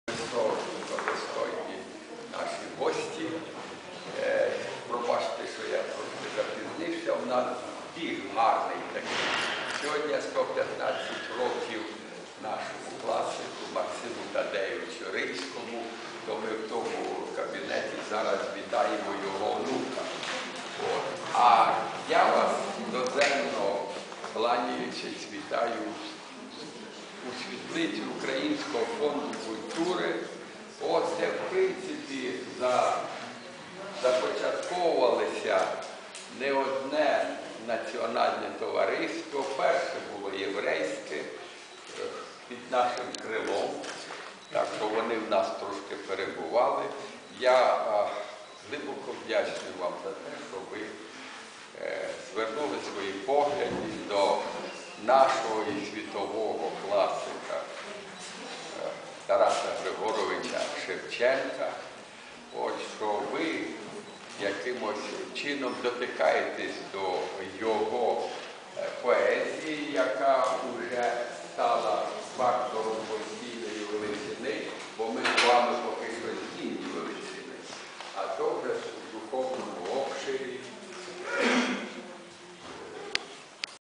Олійник Борис Բորիս Օլեյնիկ Oleinik Boris
УКРАЇНСЬКИЙ ФОНД  КУЛЬТУРИ
РАДА НАЦІОНАЛЬНИХ ТОВАРИСТВ УКРАЇНИ
19 березня 2010 р. в Українському Фонді культури відбувся вечір поезії
„ТАРАС ШЕВЧЕНКО МОВАМИ НАЦІОНАЛЬНИХ МЕНШИН. Вступне слово Бориса Олійника - поета, громадського діяча, академіка Національної академії наук України, лауреата Державної премії СРСР і Державної премії УРСР ім. Т. Г. Шевченка, Героя України.
UKRAINIAN CULTURAL FOUNDATION
Les Sociétés nationales d'Ukraine
19 mars 2010 à la Fondation ukrainienne culturels a été la soirée de la poésie
Taras Shevchenko - les langues minoritaires. Introduction Boris Oleynik - poète et homme public, académicien de l'Académie nationale des sciences d'Ukraine, URSS, lauréat du Prix d'Etat et le Prix d'Etat de l'Ukraine - eux. Shevchenko, Héros de l'Ukraine. ウクライナ文化交流基金
国立社会ウクライナ
2010年3月19日、ウクライナ文化財団の詩の夜だった
タラスシェフチェンコ - 少数民族の言語。はじめにボリスOleynik - 詩人と公共の図は、全米科学アカデミー、ウクライナ、ソビエト連邦国家賞を受賞し、国家賞をウクライナの受賞者の売春婦 - それら。シェフチェンコ、ヒーロー、ウクライナの。مؤسسة الثقافة الأوكرانية
الجمعيات الوطنية في أوكرانيا
19 مارس 2010 في مؤسسة الثقافة الأوكرانية كانت أمسية شعرية
تاراس شفتشنكو -- لغات الأقليات. مقدمة بوريس Oleynik -- وهو شاعر وشخصية عامة ، وعضو الاكاديمية الوطنية للعلوم في أوكرانيا ، والحائز على جائزة الدولة في أوكرانيا -- منهم. شيفتشينكو ، بطل اوكرانيا. UKRAINIAN CULTURAL FOUNDATION
Narodowych Stowarzyszeń UKRAINA
19 marca 2010 w ukraińskiej Fundacji Kulturalnej był wieczór poezji
Tarasa Szewczenki - języki mniejszości narodowych. Wprowadzenie Boris Oleynik - poeta i publicznej, akademik Narodowej Akademii Nauk Ukrainy, laureat Nagroda Państwowa Ukrainy - im. Shevchenko, Bohatera Ukrainy UKRAINISCHE KULTURSTIFTUNG
Die nationalen Rotkreuz-Gesellschaften UKRAINE
19. März 2010 in der ukrainischen Kulturstiftung war der Abend der Poesie
Taras Schewtschenko - der Minderheitensprachen. Einführung Boris Oleynik - ein Dichter und des öffentlichen Lebens, Akademiemitglied der Nationalen Akademie der Wissenschaften der Ukraine, Preisträger des Staatspreises der Ukraine - zu. Shevchenko, Held der Ukraine. אוקראינית קרן התרבות
החברות הלאומי באוקראינה
19 מרס 2010 של קרן התרבות האוקראיני היה ערב של שירה
Taras Shevchenko - שפות מיעוט. מבוא בוריס Oleynik - משורר ואיש ציבור, אקדמיה של האקדמיה הלאומית למדעים של אוקראינה, חתן פרס המדינה של אוקראינה - אותם. שבצ 'נקו, גיבור אוקראינה.